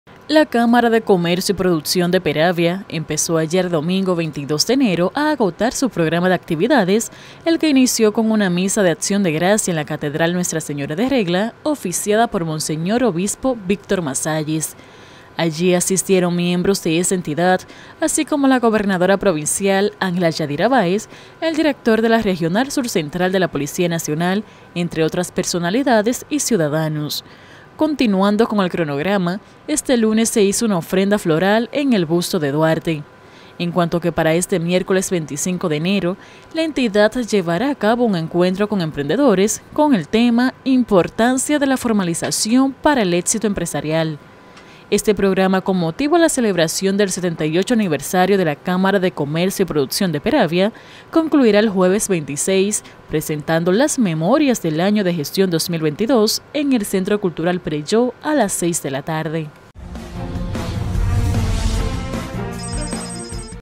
Cámara de Comercio y Producción de Peravia celebra 78 aniversario
La Cámara de Comercio y Producción de la Provincia Peravia celebra su 78 aniversario de fundación, y con motivo a esto, ha creado una agenda de actividades.